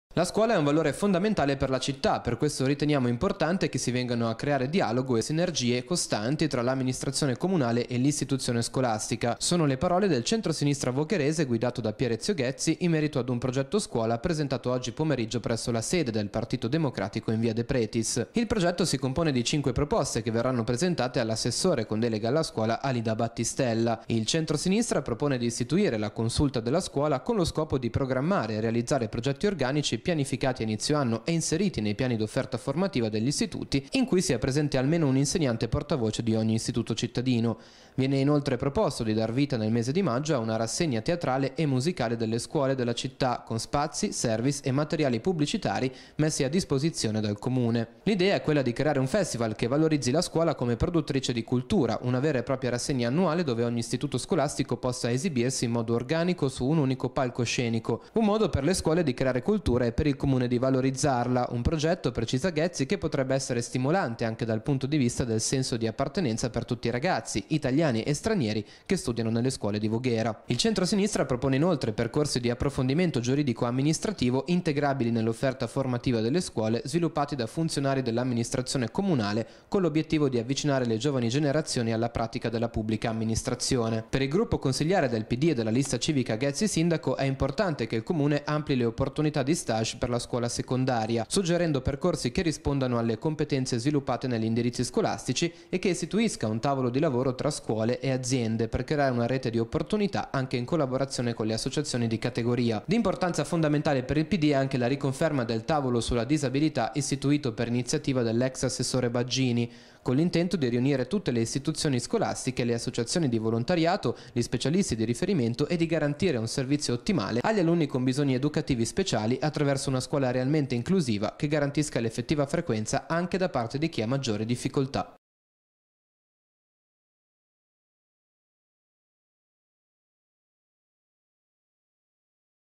Presentato il "progetto scuola" del centrosinistra vogherese
Servizio dell'8 settembre 2015

A cura di: Marco Cignoli
Tecnico: Matteo Garifo
Produzione: Jab Media